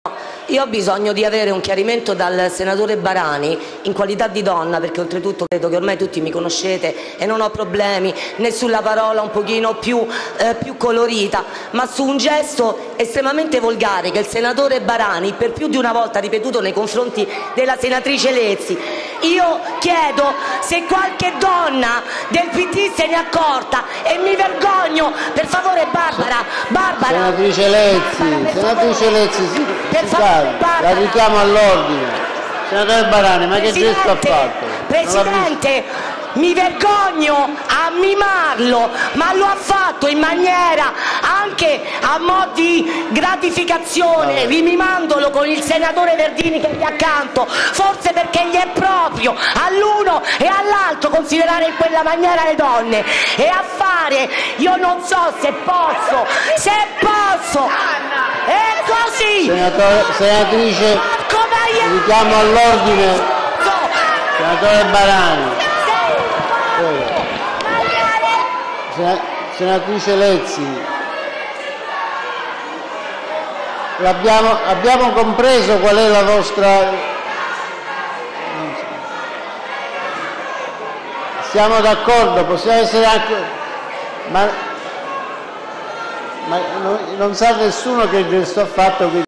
BARANI ACCUSATO DI GESTO OSCENO AL SENATO, TAVERNA (M5S): SEI UN MAIALE
(Agenzia VISTA) - Roma, 2 Ottobre 2015 - La senatrice M5S Paola Taverna contro il verdiniano Barani, accusato dagli esponenti del Movimento 5 Stelle di aver mimato il gesto del sesso orale nei confronti della grillina Barbara Lezzi. Mentre Barani nega tutto, il Presidente del Senato Grasso ordina uno stop ai lavori per comprendere come si siano svolti i fatti. / DA DIRETTA SENATO